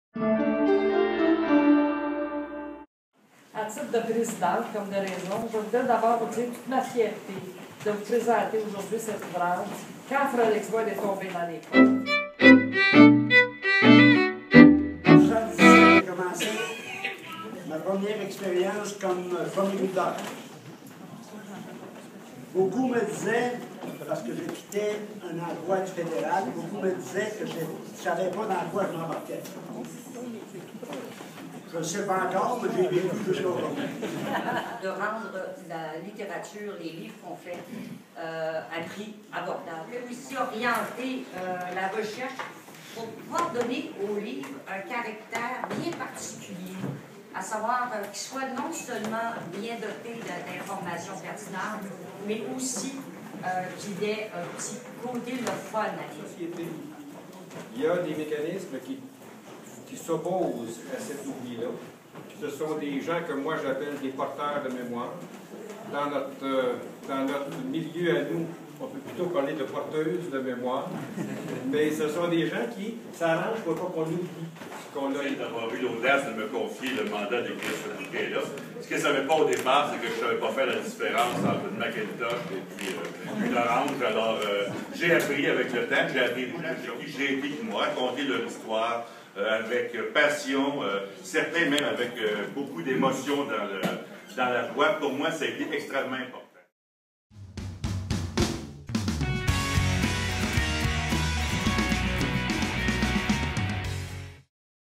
Tombé dans les pommes
Lancement du livre sur l'histoire de la pomiculture à Frelighsburg.